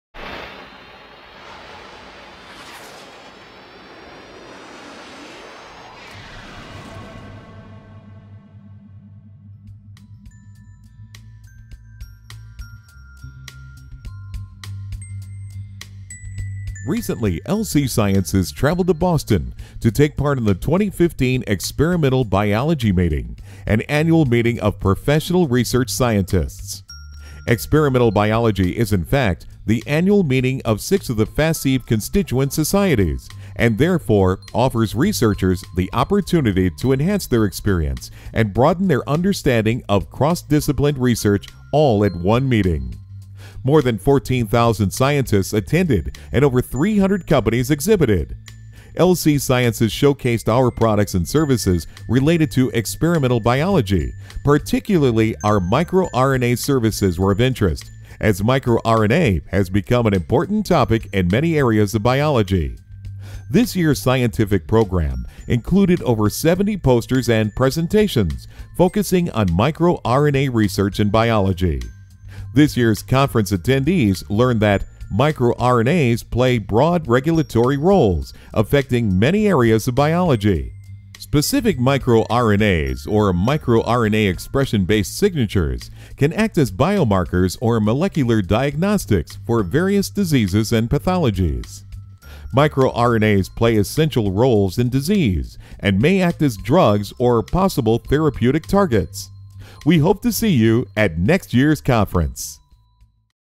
Experimental Biology 2015
Recently, LC Sciences traveled to Boston to take part in the 2015 Experimental Biology Meeting - an annual meeting of professional research scientists.

Experimental Biology is in fact, the annual meeting of six of the Faseb constituent societies, and therefore offers researchers the opportunity to enhance their experience and broaden their understanding of cross-disciplined research, all at one meeting.

More than 14,000 scientists attended and over 300 companies exhibited.

LC Sciences showcased our products and services related to experimental biology, particularly our micro-RNA services were of interest, as micro-RNA has become an important topic in many areas of biology.

This year’s scientific program included over 70 posters and presentations focusing on micro-RNA research in biology.

This year’s conference attendees learned that:

• Micro-RNAs play broad regulatory roles, affecting many areas of biology
• Specific Micro-RNAs or Micro-RNA expression based signatures can act as biomarkers or molecular diagnostics for various diseases and pathologies.
• Micro-RNAs play essential roles in disease and may act as drugs or possible therapeutic targets.

We hope to see you at next year’s conference!